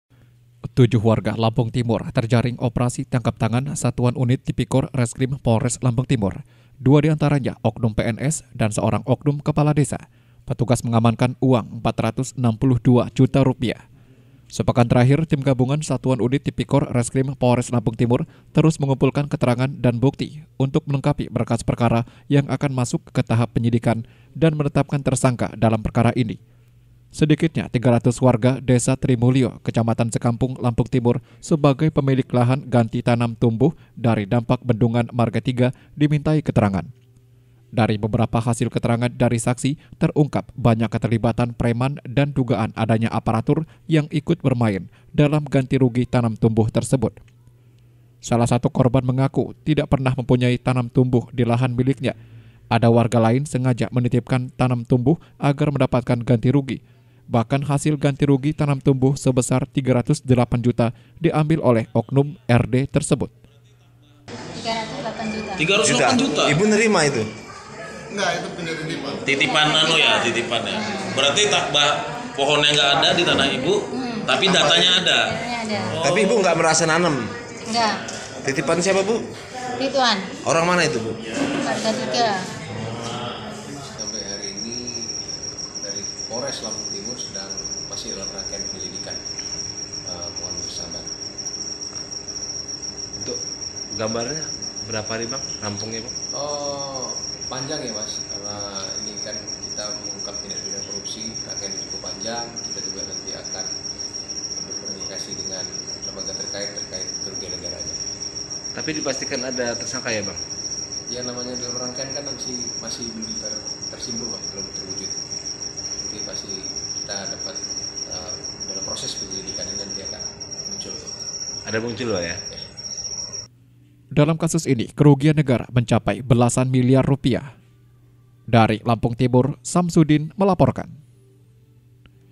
Korupsi Bendungan Marga Tiga, Negara Rugi Belasan Miliar Rupiah
Perkara  Operasi Tangkap Tangan Melibatkan Tujuh Warga Lampung Timur Dua Diantara Nya Oknum PNS Dan Oknum Kepala Desa Atas Dugaan  Ganti Rugi Tanam Tumbuh Bendungan Marga Tiga Terus Berlanjut Diduga Dalam Kasus Ini Melibatkan Banyak Oknum 300 Warga Pemilik Lahan Telah Diperiksa.